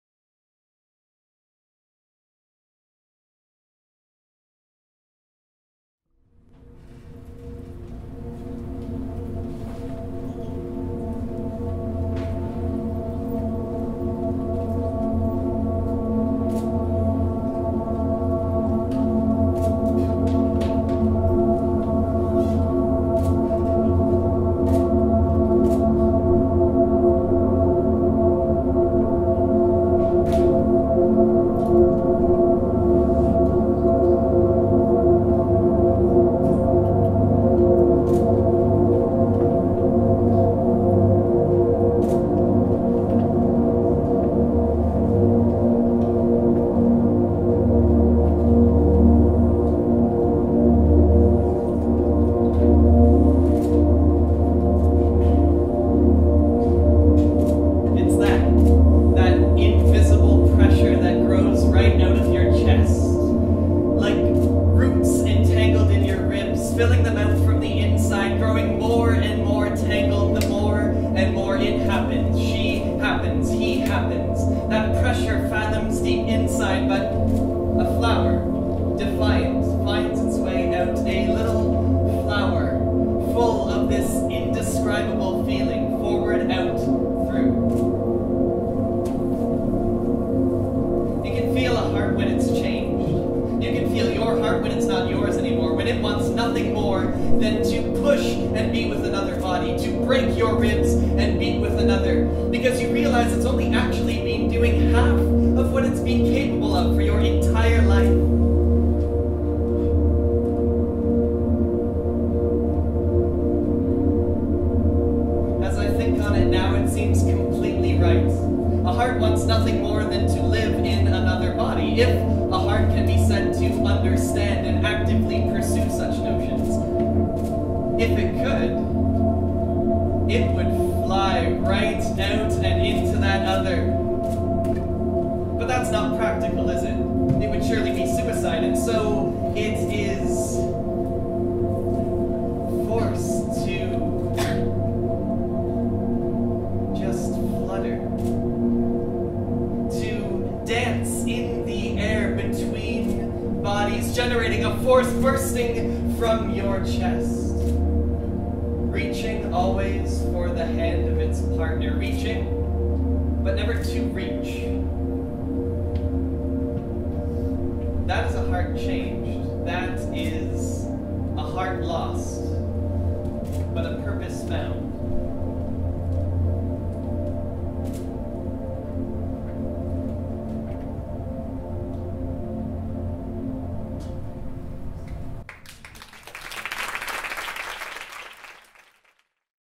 COLLAGE: Synergy 2015 | Bare
Collaborators: Tavia Christina, Josh Cezar (dancers) & Andrew Gaboury (spoken word)

This is a truly collaborative piece with Andrew doing spoken word, alongside Josh and Tavia dancing to his words and music. It's a memory, dream-like piece, where there is a rekindling memory that Josh inflicts, and Andrew is more of the storyteller.

COLLAGE: Synergy Dance Showcase 2015
Produced by: MiCha Entertainment